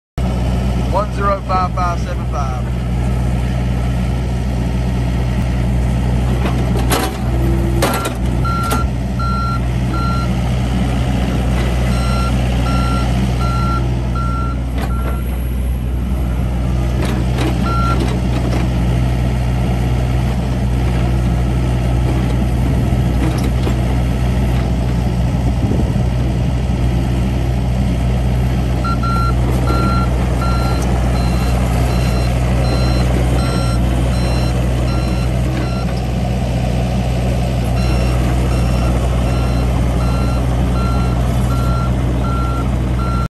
2014 DEERE 60G For Sale
RUBBER TRACK, EROPS, PUSH BLADE, 36" BUCKET W/ QUICK COUPLER, HYD THUMB, AUX VALVES, SN-1FF060GXEDJ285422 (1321 HOURS SHOWING)

 [2014 JOHN DEERE 60G EXCAVATOR]